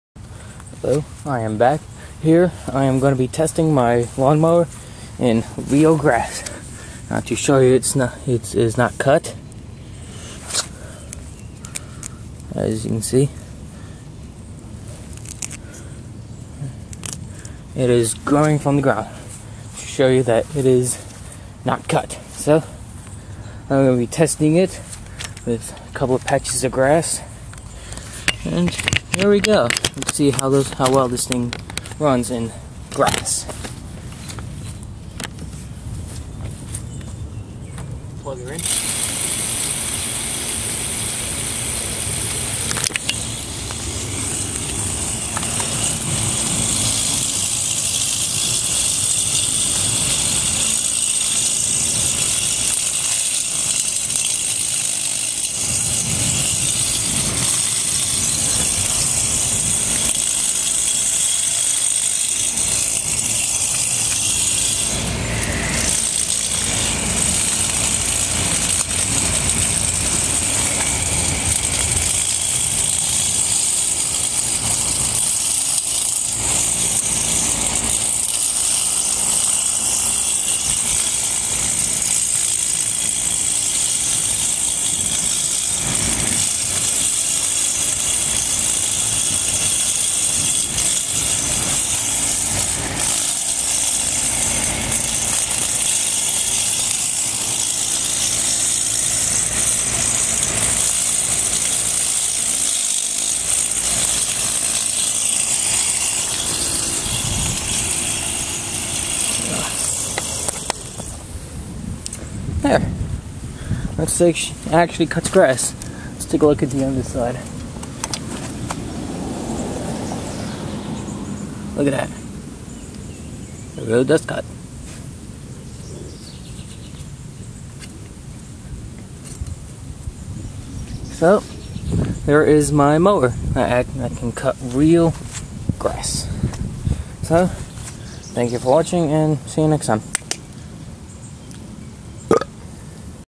Home made Knex Lawn Mower 2.0 lawn test
well see if this can really cut grass just like a real electric/gas mower
note: not for kids due to high voltage and a dangerous spinning blade can cause severe cuts